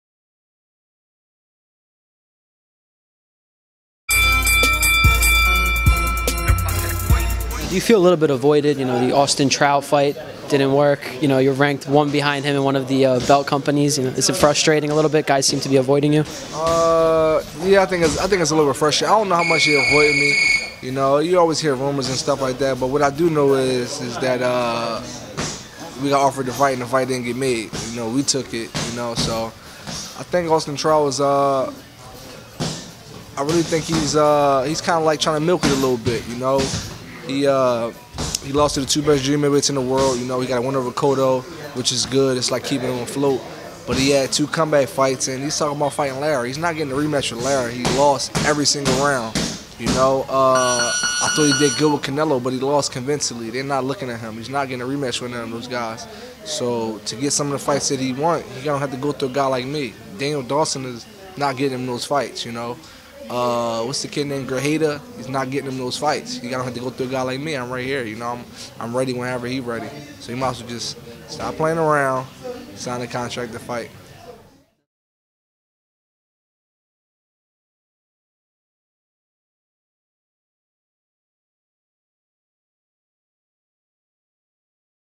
Julian Williams Sends Message, Puts Austin Trout On Notice
Joshua Grayer talks to Julian Williams and talks details as to why the fight hasn't happen.